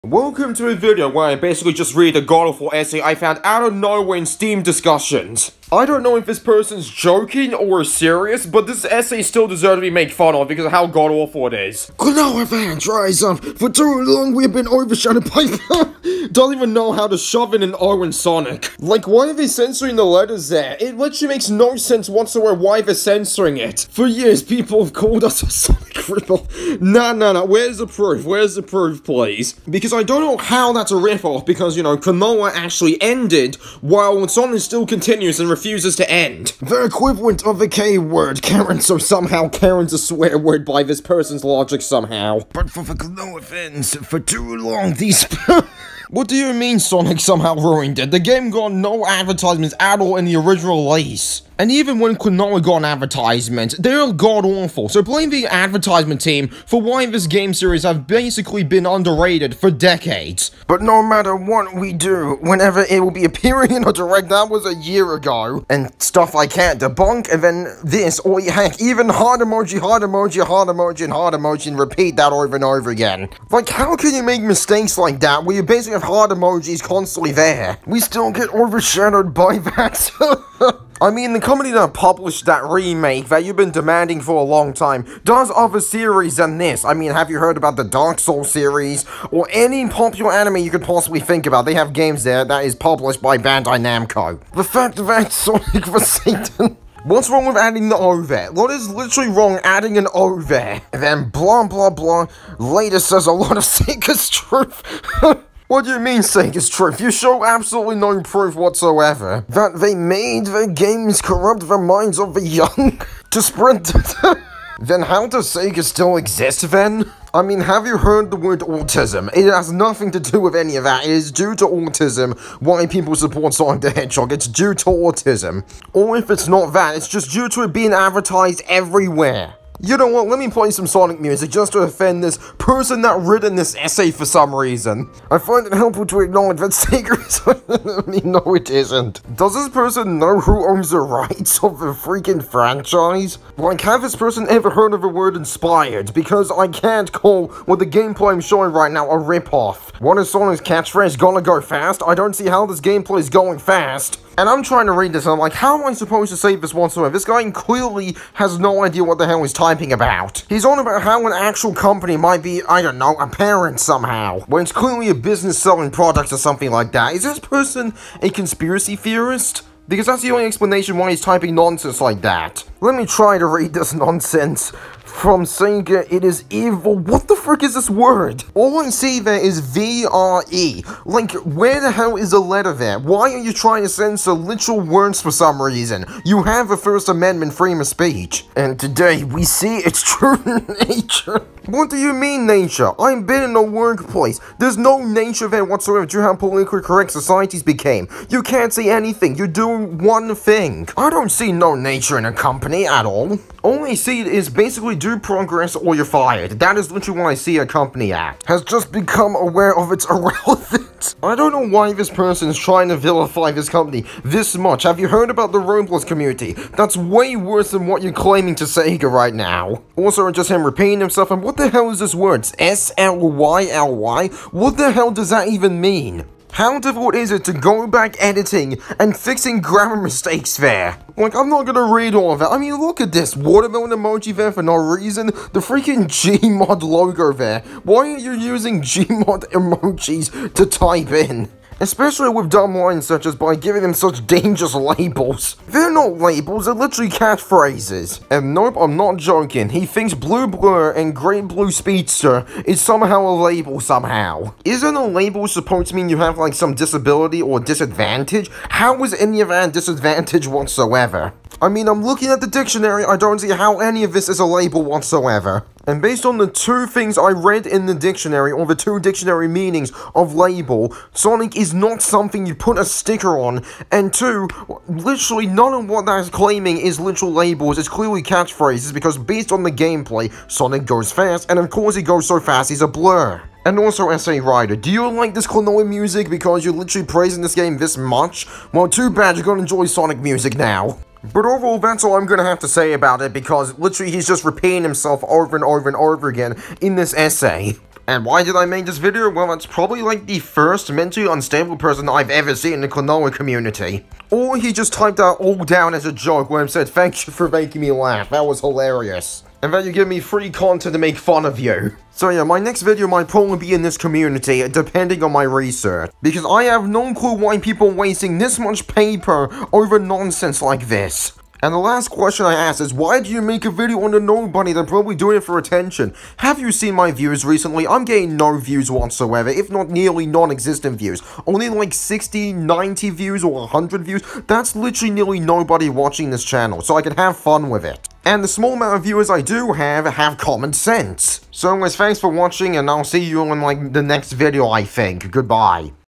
Reading A Terrible Essay
Basically found this out of no where on steam, so I thought of an idea of, I should just make a video on this, because even if this person typed this stuff as a joke, it’s still a terrible essay where some people will say, “not gonna read all that” I tried reading it, and I basically give up on it, because he kept repeating himself to much.